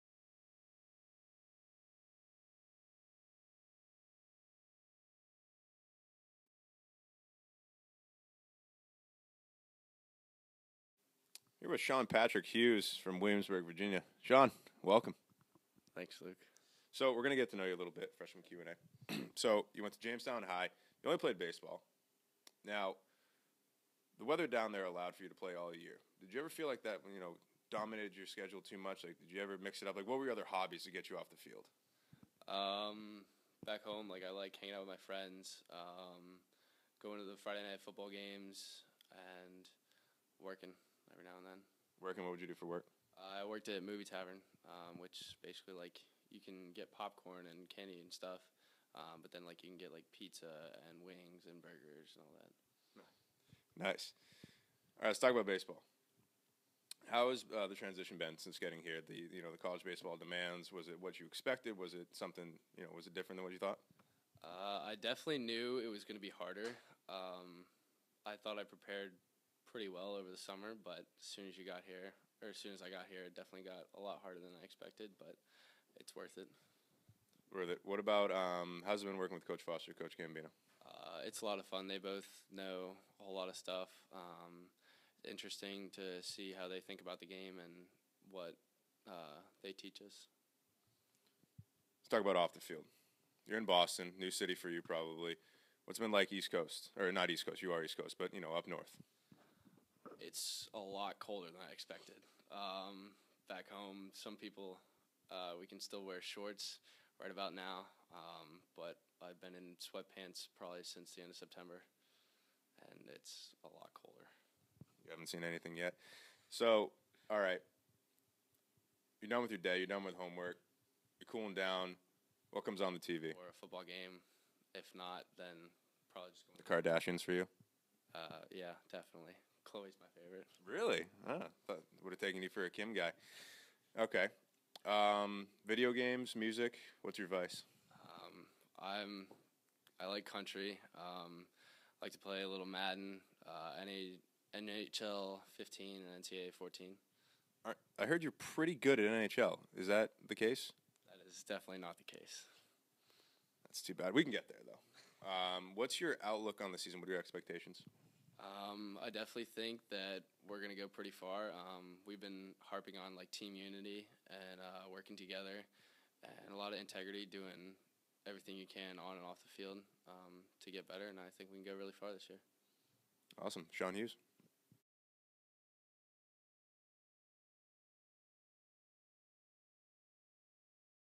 Birdball Freshman Q&A: Sean Hughes
The Boston College baseball team is comprised of 13 freshmen and junior Luke Fernandes is catching up with all of them. This week, he spoke to right-handed pitcher Sean Hughes from Williamsburg, Va., about getting used to collegiate ball on the field and what Sean does off the field for fun.